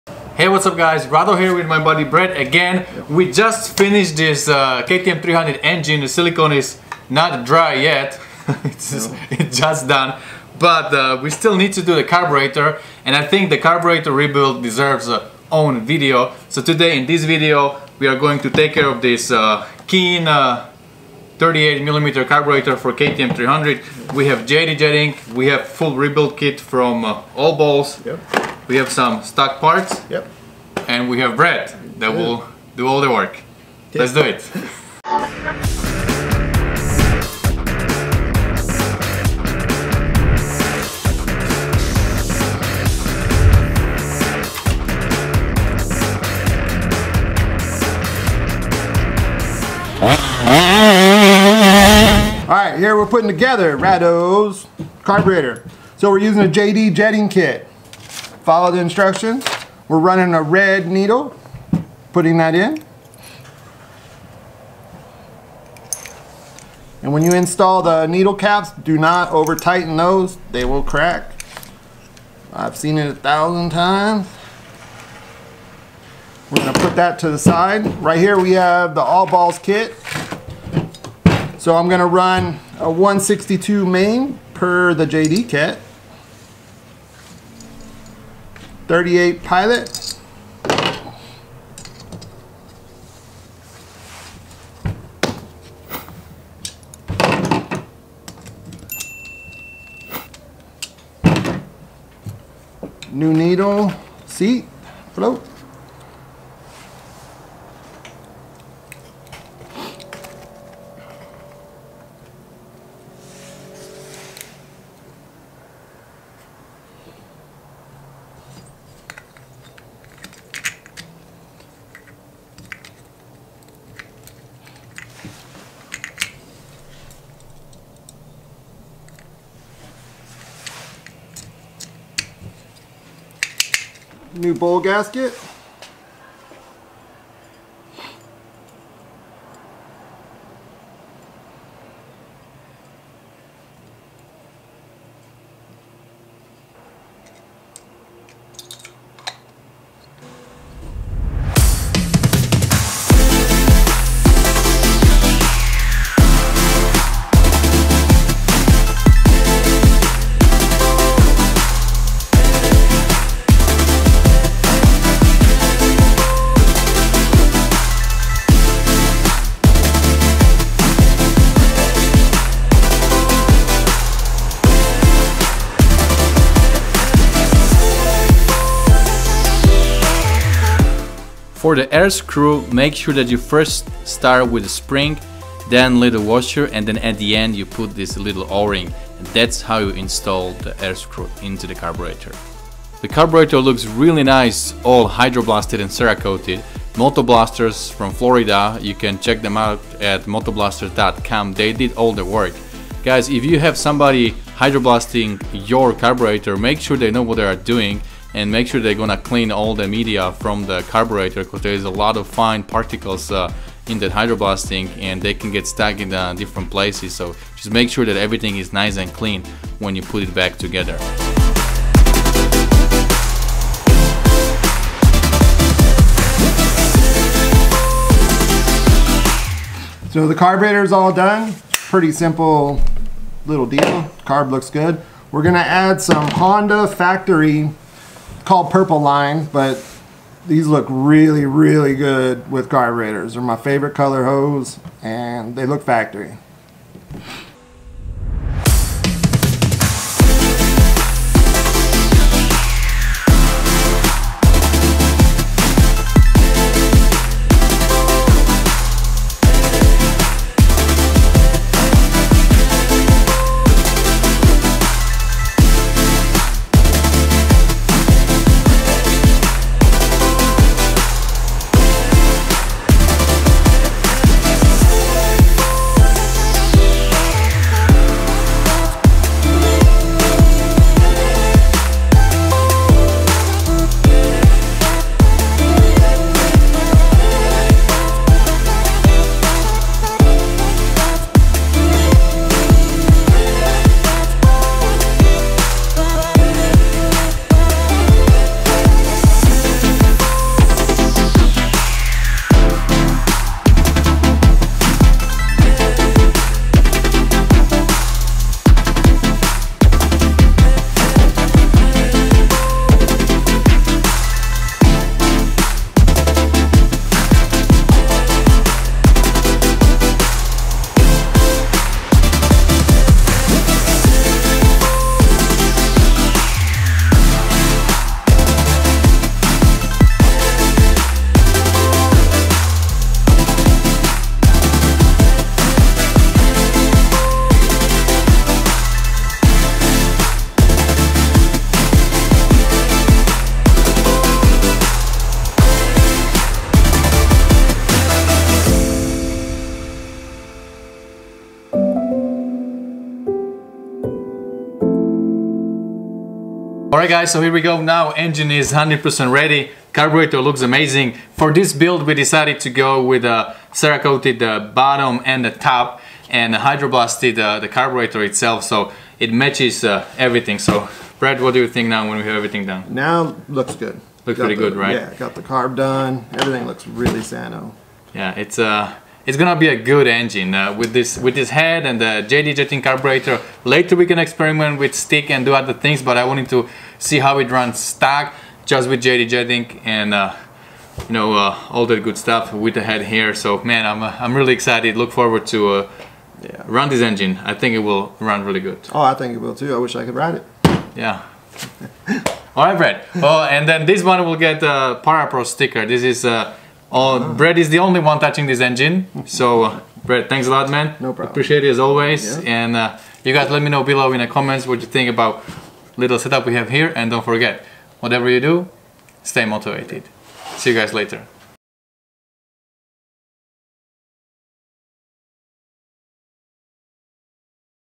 How to rebuild 2stroke Keihin PWK carburetor - KTM 300XC build
hey guys, in this video we will show you how to rebuild 2 stroke Keihin PWK 38mm carburetor on your 2 stroke dirt bike. We are going to use all balls rebuild kit and JD jetting kit for the jetting and needle. This Keihin carb is for our 2 stroke dirt bike KTM 300 XC build.

things I use and recommend:
my air filter care
my engine oil 2stroke/4stroke
my hour meter
my body armor
my knee guards
my boots
my handguards
my armpump solution
my camera lens
my camera

how to rebuild keihin carburetor